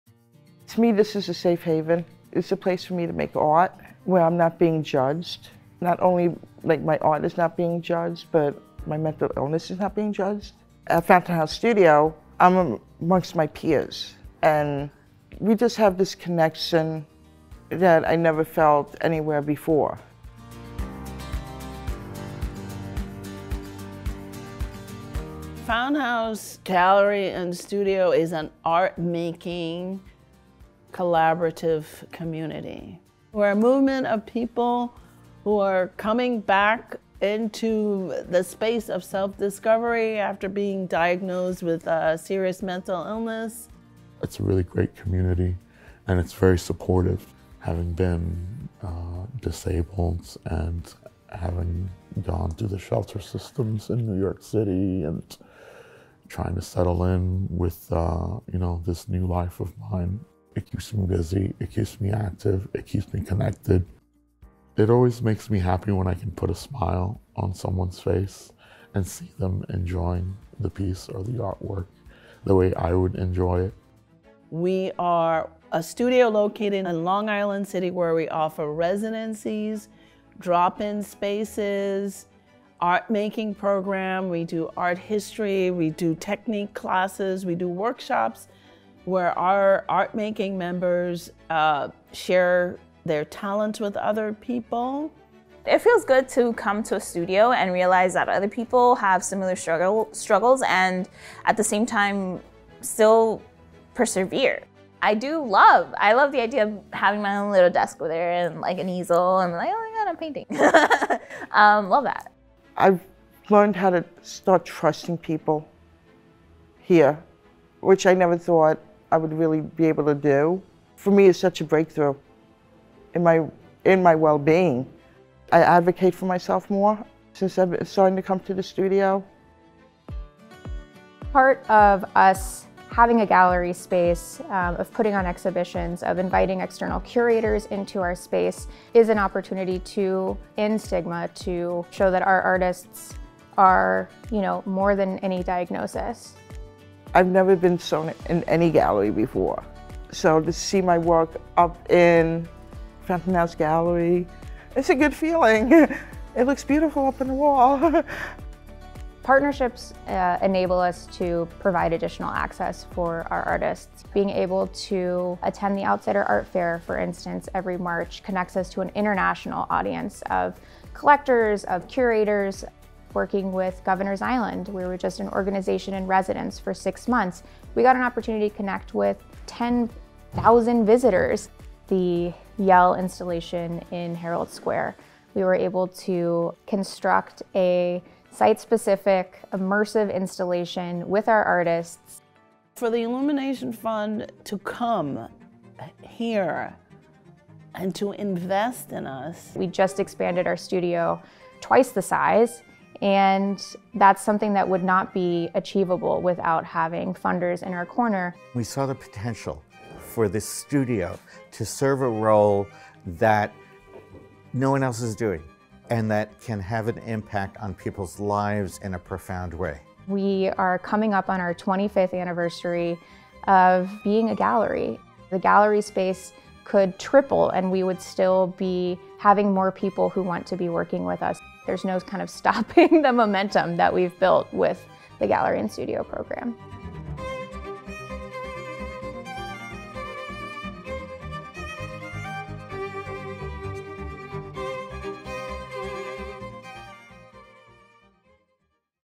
Fountain House Gallery & Studio
Fountain House Gallery and Studio supports the careers and creative visions of contemporary artists living with mental illness.

Video created by The Laurie M. Tisch Illumination Fund's Arts in Health initiative, which supports organizations that use the arts to address health issues, with a particular focus on mental health stigma, trauma, and age-related disease. Fountain House Studio and Gallery is a grantee of the Laurie M. Tisch Illumination Fund.